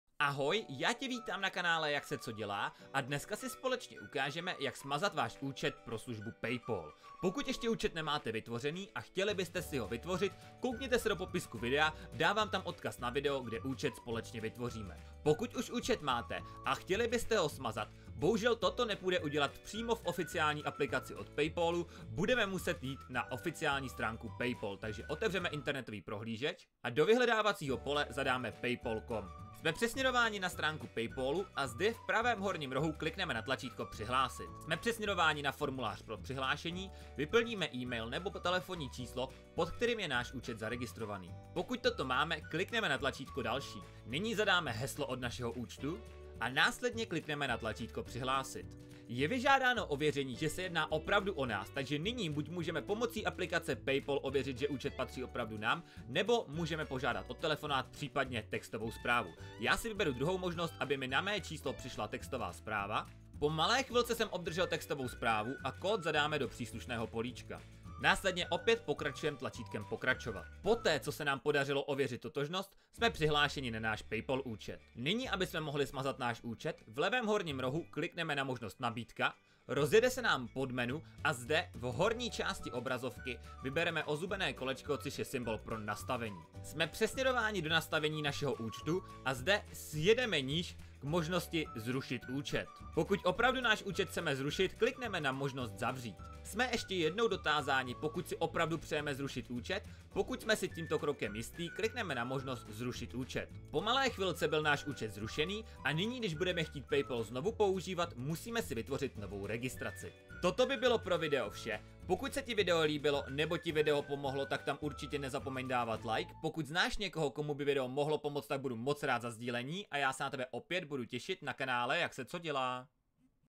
Jak ZRUŠIT PAYPAL ÚČET | Návod | Mobil / PayPal
► Podpoř tvorbu nákupem na Alza.cz 🛒 Ve videu si ukážeme jak krok po kroku smazat váš PayPal účet pokud ho už dále nechcete používat.